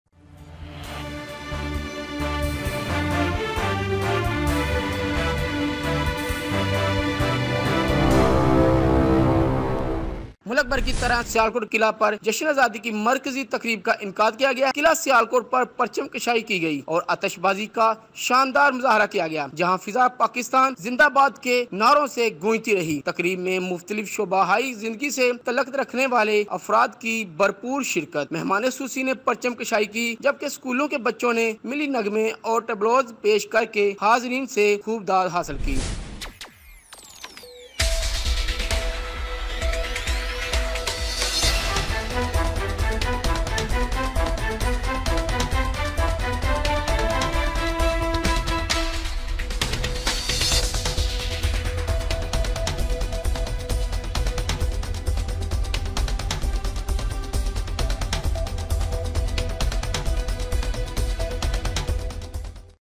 14th August Independence Day Celebrations Held At Fort Sialkot┃PAKasiaTV
سیالکوٹ: (یاسر رفیق، میڈیا رپورٹر) ــ  قلعہ سیالکوٹ پر پرچم کشائی کی گئی اور آتشبازی کا شاندار مظاہرہ کیا گیا جہاں فضا پاکستان زندہ باد کے نعروں سے گونج اٹھی۔

تقریب میں مختلف شعبہ ہائے زندگی سے تعلق رکھنے والے افراد کی بھر پور شرکت مہمانان خصوصی نے پرچم کشائی کی، جبکہ سکول کے بچوں نے ملی نغمے اور ٹیبلوز پیش کر کے حاضرین سے خوب داد وصول کی۔

About PAKasia Web TV
Pakasia Web TV is the best platform to know about the latest breaking news and video updates worldwide.